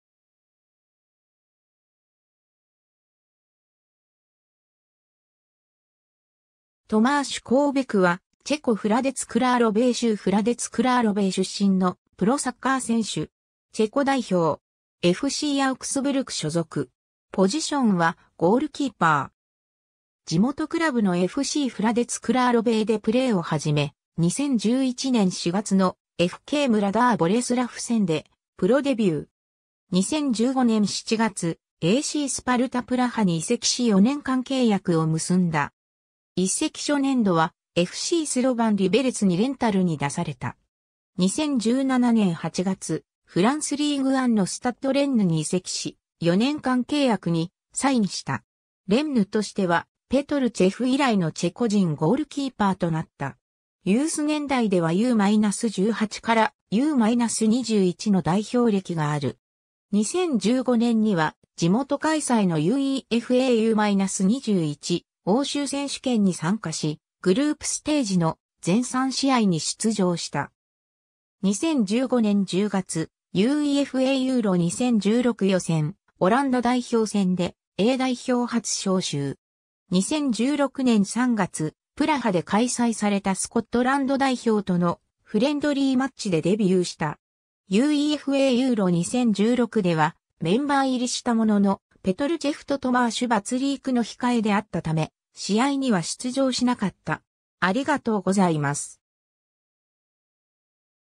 トマーシュ・コウベク
FCフラデツ・クラーロヴェーの選手
ACスパルタ・プラハの選手
FCスロヴァン・リベレツの選手
スタッド・レンヌFCの選手
フラデツ・クラーロヴェー出身の人物
トマーシュ・コウベク（Tomáš Koubek、1992年8月26日 - ）は、チェコ・フラデツ・クラーロヴェー州フラデツ・クラーロヴェー出身のプロサッカー選手。
チェコ代表。
FCアウクスブルク所属。
ポジションは、ゴールキーパー。
 地元クラブのFCフラデツ・クラーロヴェーでプレーを始め、2011年4月のFKムラダー・ボレスラフ戦でプロデビュー。
 2015年7月、ACスパルタ・プラハに移籍し4年間契約を結んだ。
移籍初年度はFCスロヴァン・リベレツにレンタルに出された。
 2017年8月、フランス・リーグ・アンのスタッド・レンヌに移籍し、4年間契約にサインした。
レンヌとしてはペトル・チェフ以来のチェコ人ゴールキーパーとなった。
 ユース年代ではU-18からU-21の代表歴がある。
2015年には地元開催のUEFA U-21欧州選手権に参加し、グループステージの全3試合に出場した。
 2015年10月、UEFA EURO 2016予選・オランダ代表戦でA代表初招集。
2016年3月、プラハで開催されたスコットランド代表とのフレンドリーマッチでデビューした。
 UEFA EURO 2016ではメンバー入りしたものの、ペトル・チェフとトマーシュ・ヴァツリークの控えであったため、試合には出場しなかった。